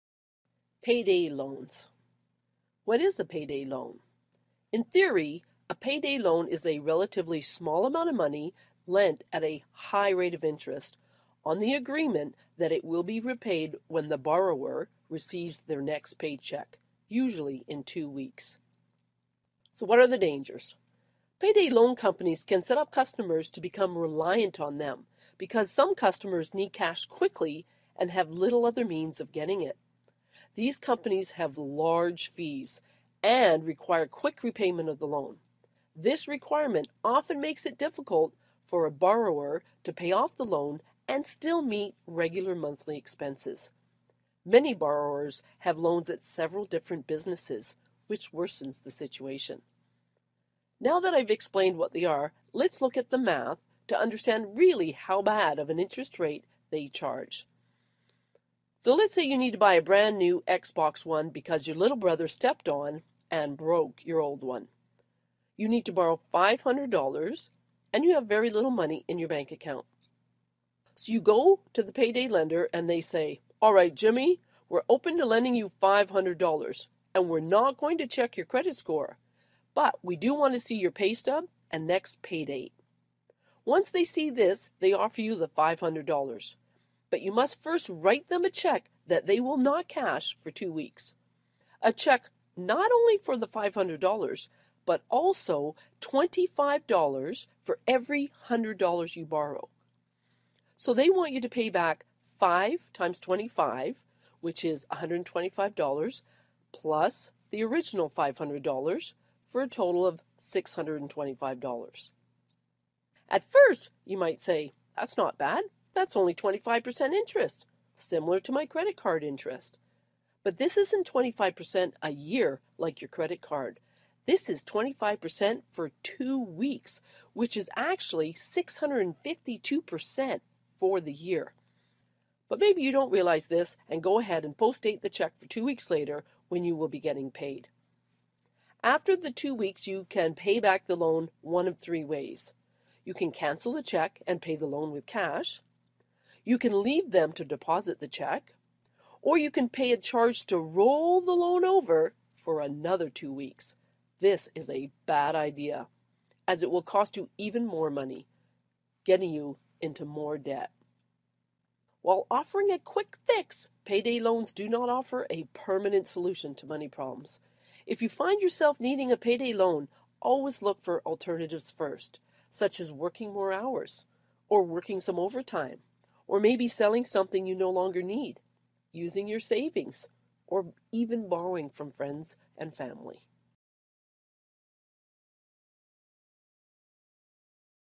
WCLN - Math - Payday Loans
Description of Payday loans and their associated costs.